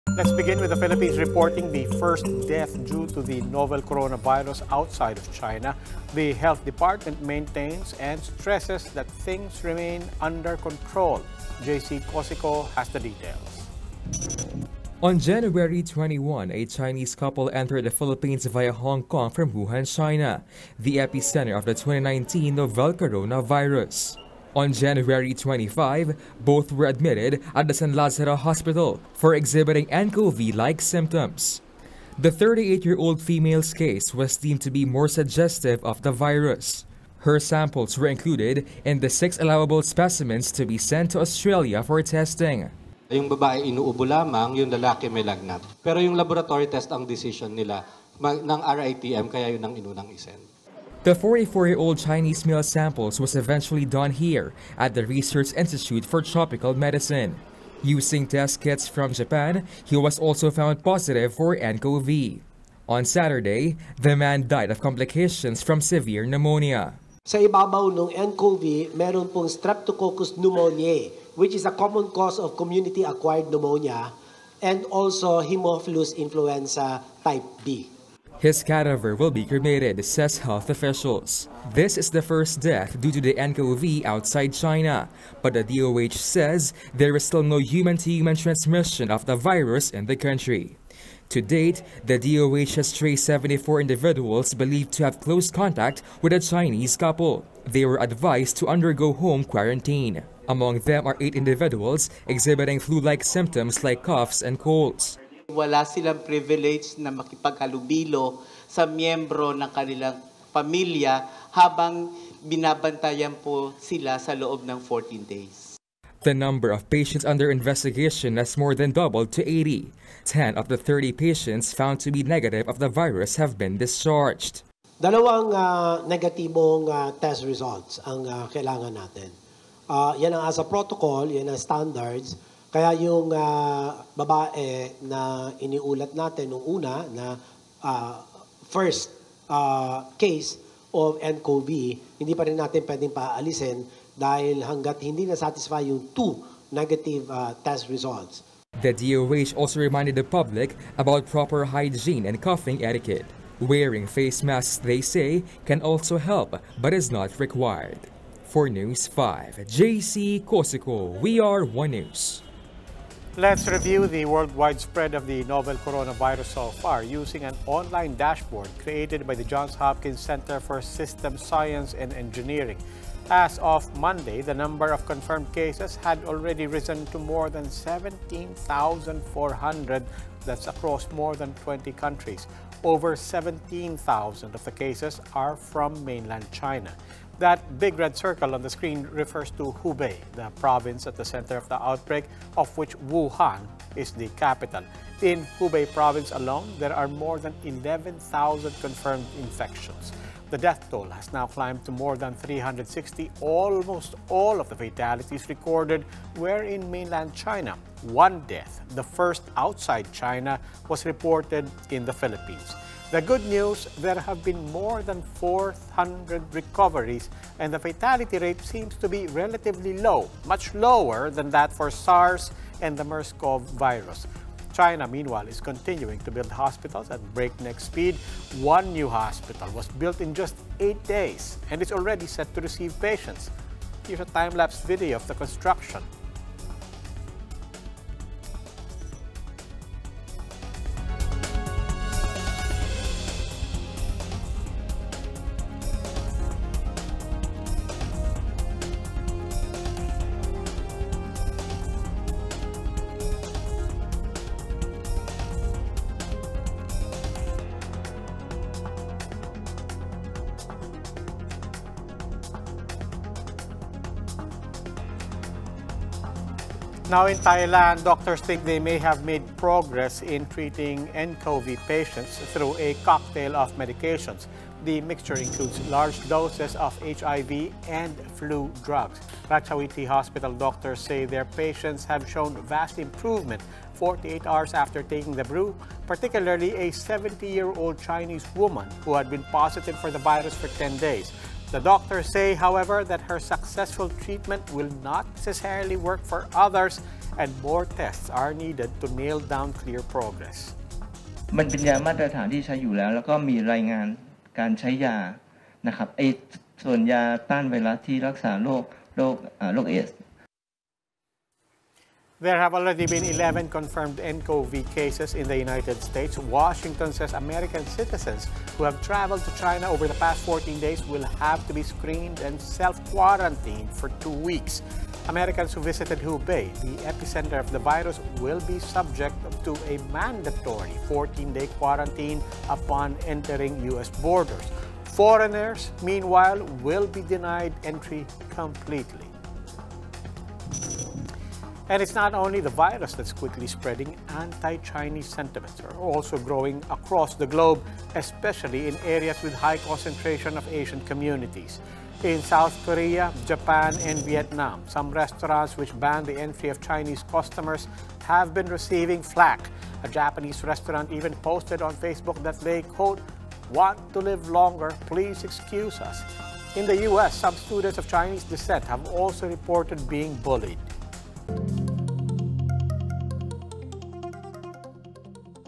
DOH reports first death from novel coronavirus outside China in PH
Philippine health officials say the death of a novel coronavirus patient in the country was the first outside China. But they stress that things remain under control.